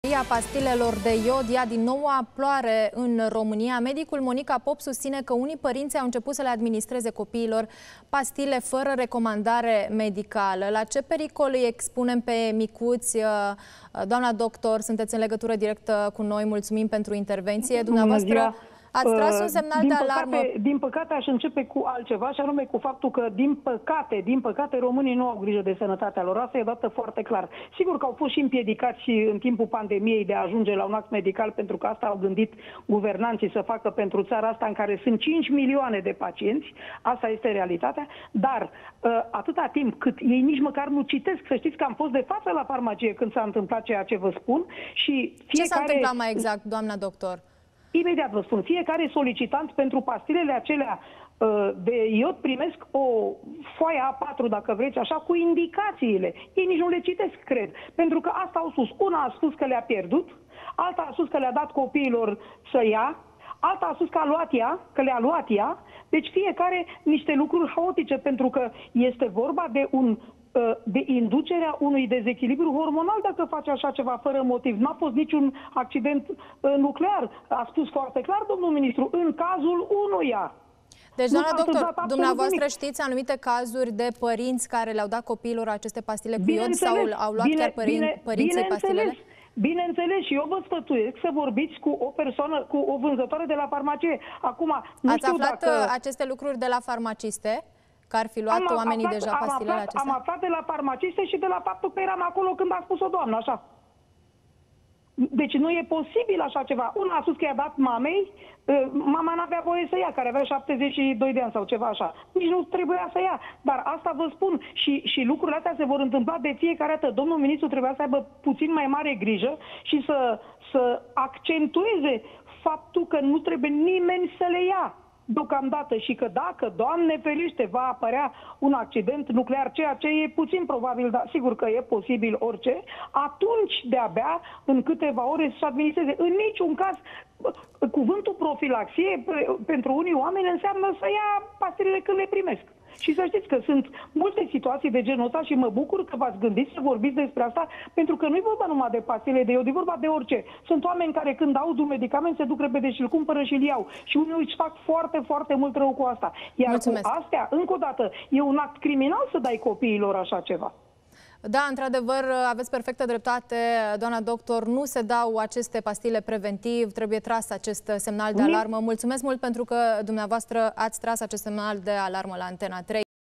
Monica Pop despre isteria pastilelor de iod
Isteria pastilelor de iod ia din nou proportii. Medicul Monica Pop susţine că unii părinţi au inceput sa le administreze copiilor pastile făra recomandare medicala. La ce pericol îi expun pe micuţi.